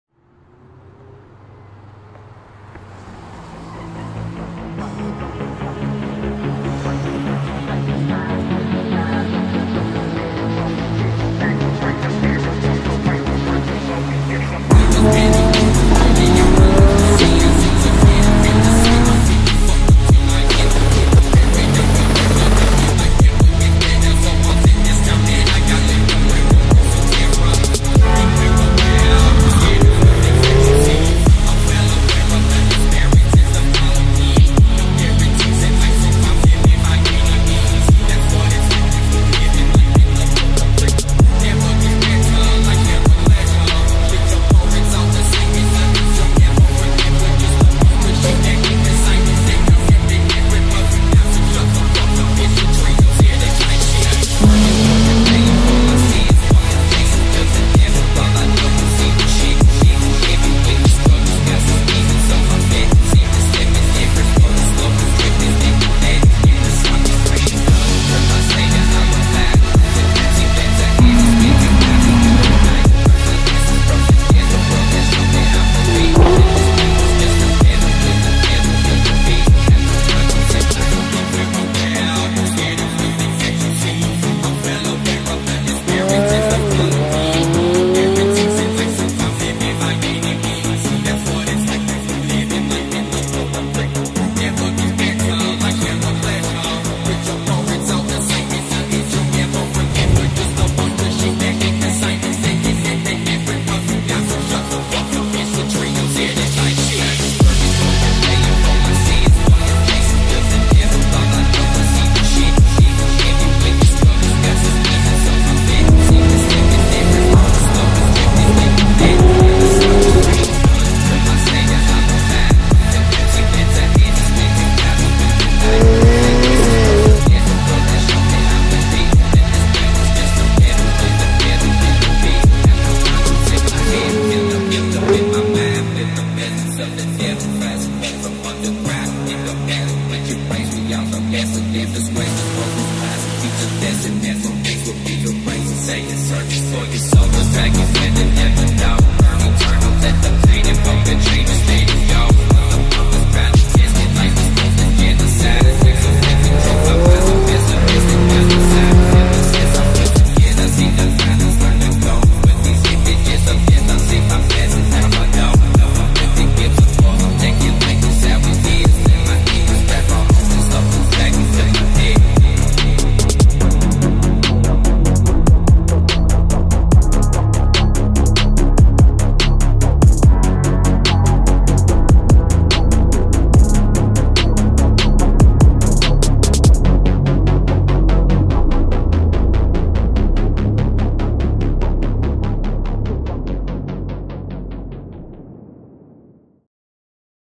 Corsa Italia 2020 Drive Trailer | The Drivers Club
Impressionen der Corsa Italia 2020 - dem jährlichen Highlight der TDC Saison. Auch für 2021 hat The Drivers Club mit der Corsa Di Lusso sowie der Corsa Alpine einzigartige Events mit und für Fahrer reinrassiger Supersportwagen entworfen.